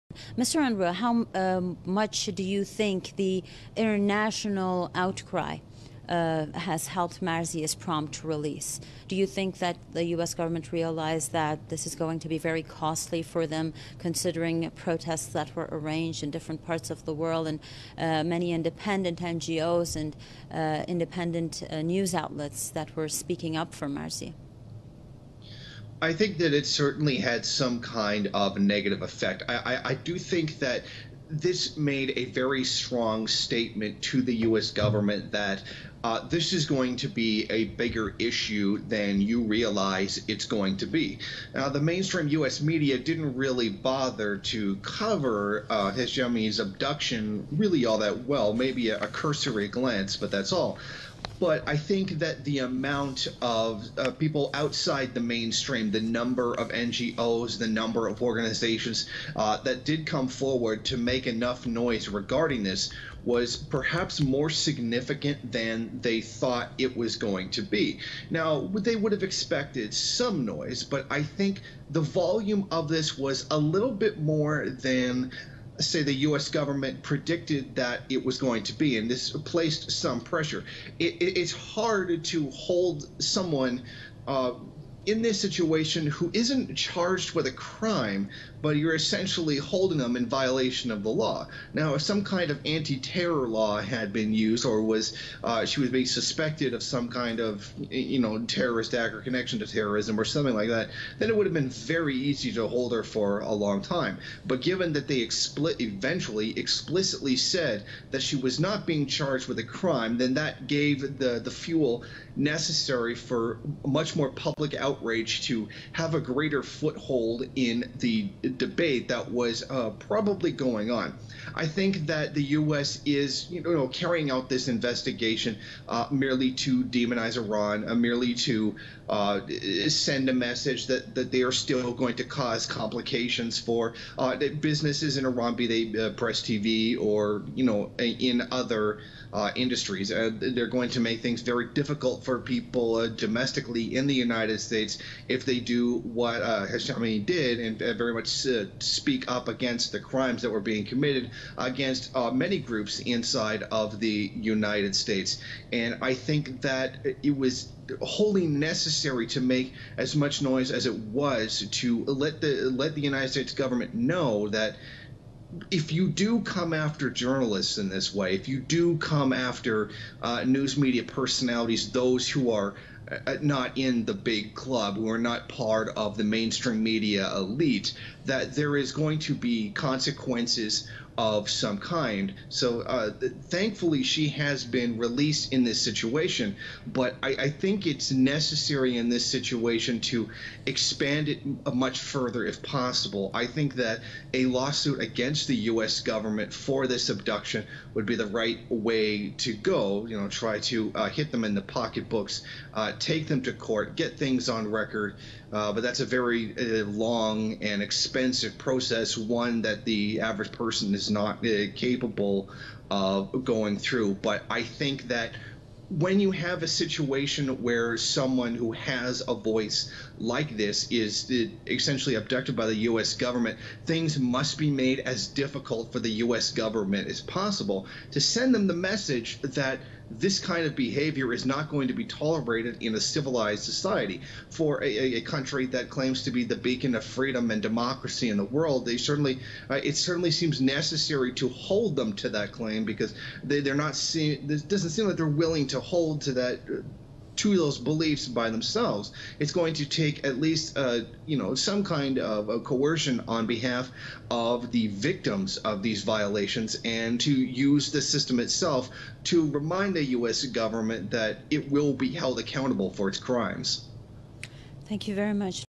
Press TV's journalist Marzieh Hashemi released from US jail after 10 days
Source: Press TV